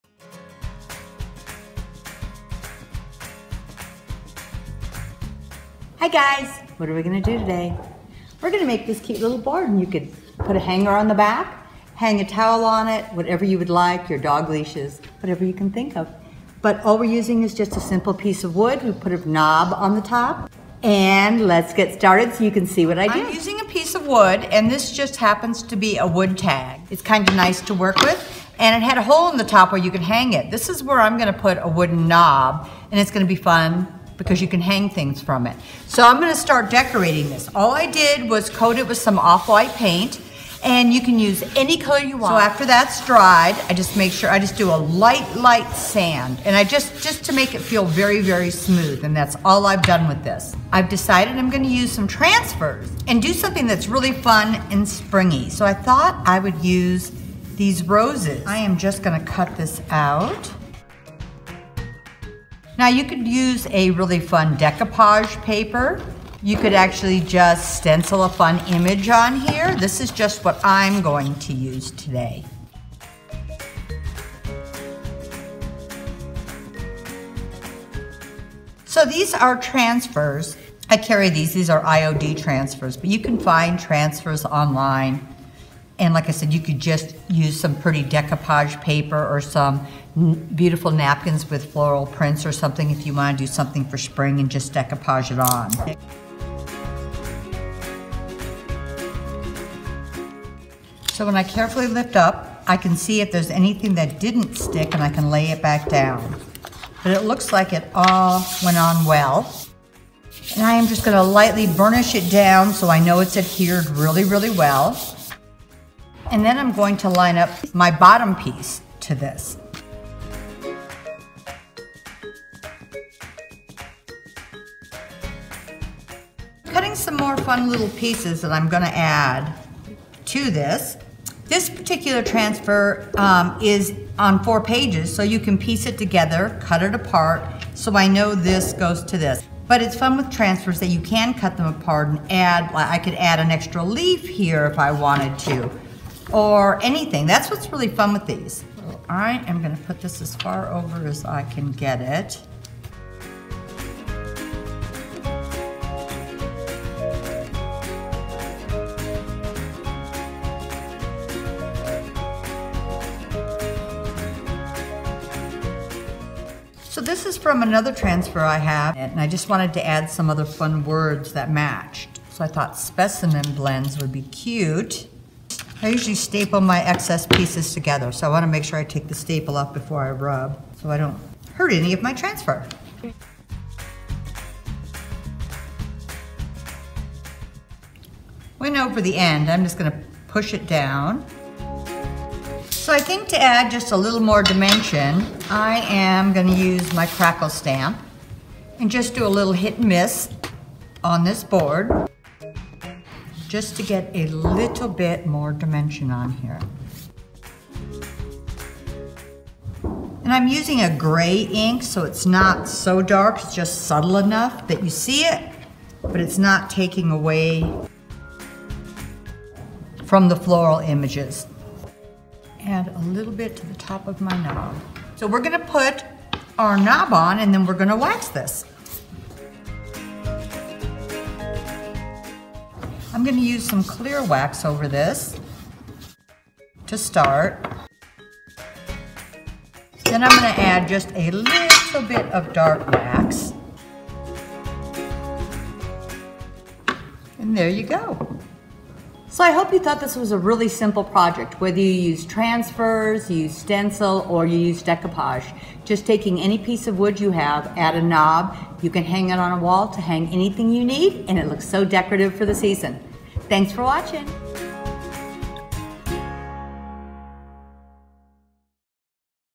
Carmen's Curious Tutorial: Springtime Knob Board
Using a wood board (I used my tag shaped boards) and a wood knob I created this fun springtime board to hand items from (jewelry, keys, leashes, towels, etc.).
I also used my IOD transfers, but you can use any fun images! Transfers, stamps, decoupage, stencils and more.